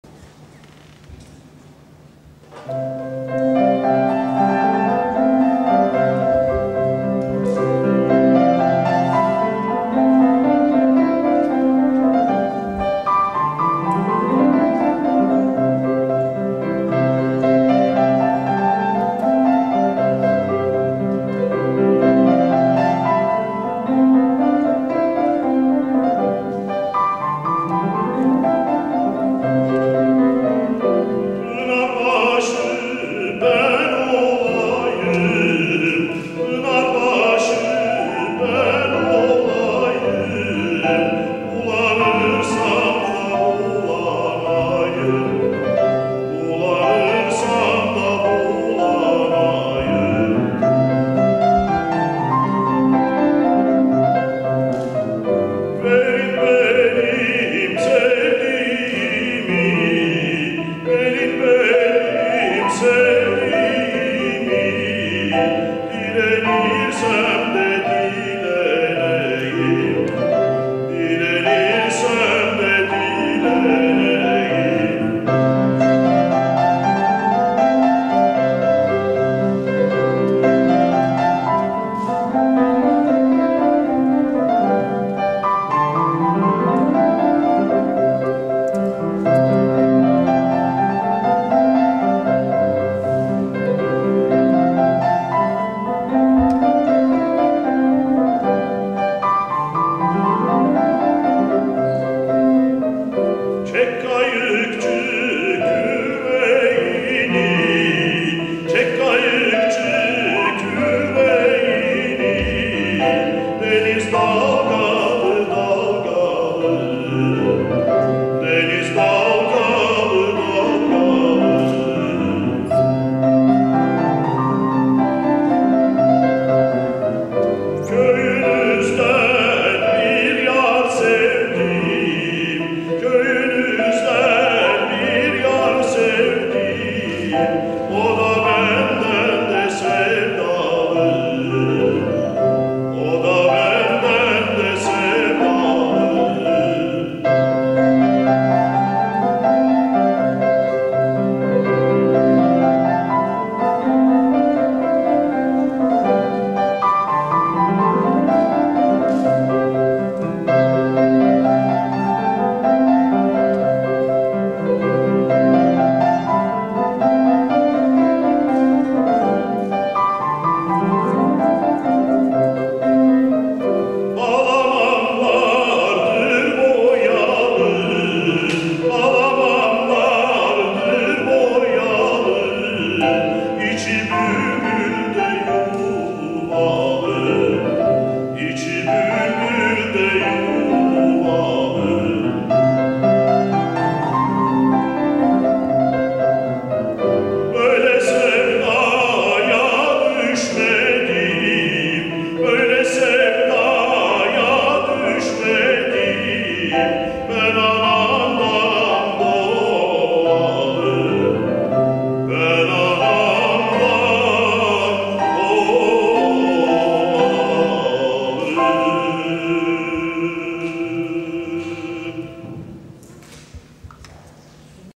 11. MUMF 2. BESTE YARIŞMASI BİRİNCİSİ
Rumuz OZAN
Yalçın Özgüngör

Aydın Karlıbel, piyano
Hasan Alptekin, solist
Zeynep Tatlıpınar Kağnıcı, solist
Işıl Cavga, solist